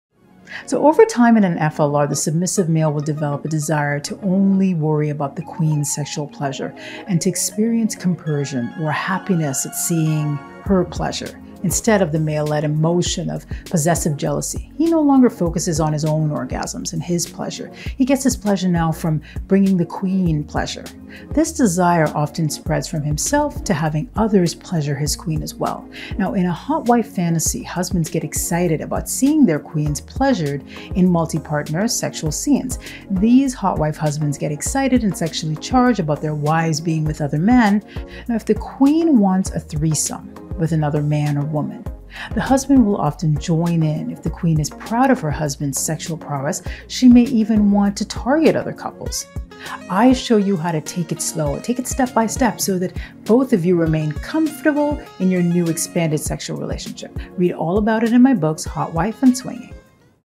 Hotwife
The passion in a marriage can evolve. Keep the flame alive with a hotwife romance of your wife. Men in FLR become fluent in their wife’s love language. When you as a husband tell your wife she's hot she becomes hotter. Men who tell their wives they’re hot and believe it with conviction express their love in a modern open way and this equals a new romance. It’s win-win. You must have been pretty good at dating your wife or she wouldn’t have married you! So why stop now? Have you considered one hotwife date a month and opening up your relationship? There are many ways to stay romantic and making your wife a Hotwife can make all the difference in your relationship.